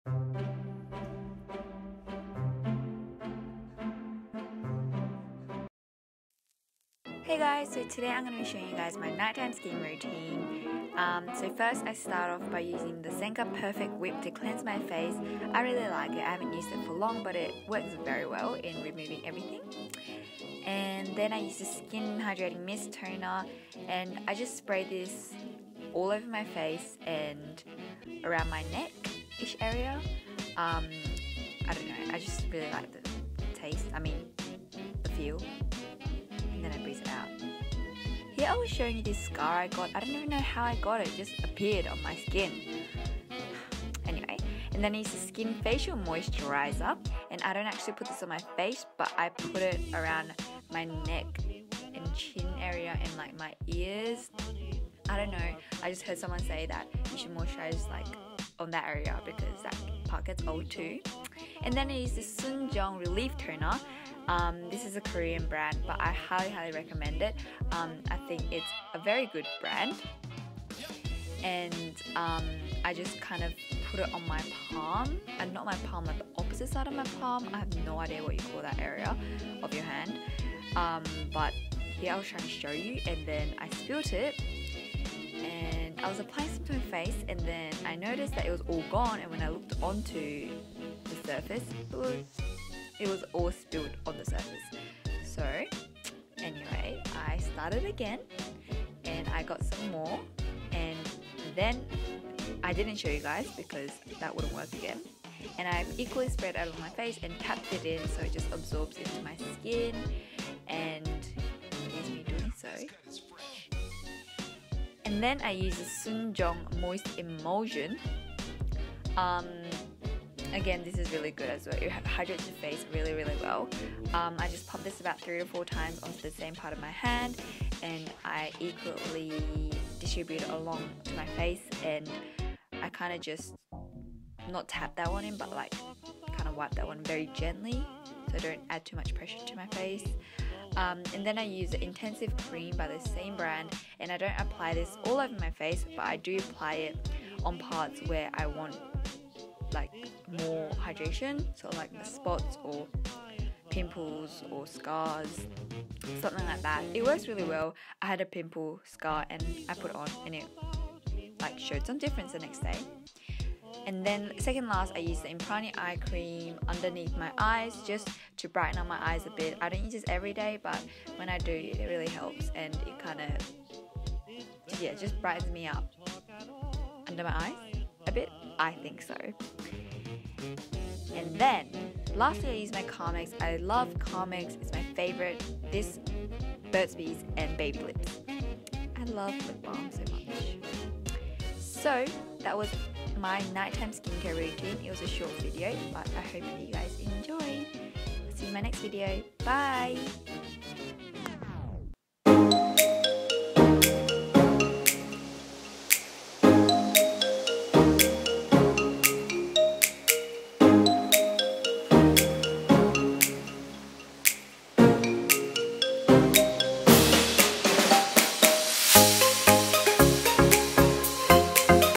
16 year old Skincare Routine! (Korean products)
hiiiiiiii :))))

in this video, I show you guys my everyday (nighttime) skincare routine :D I generally have good skin so hopefully this will help someone (?)

Products used:
1. Senka - Perfect whip
2. Sukin - Hydrating mist
3. Sukin - Facial moisturiser
4. SoonJung - Relief toner
5. SoonJung - Moist emulsion
6. SoonJung - Intensive cream
7. Enprani - Aqua eye cream
8. Carmex lip balm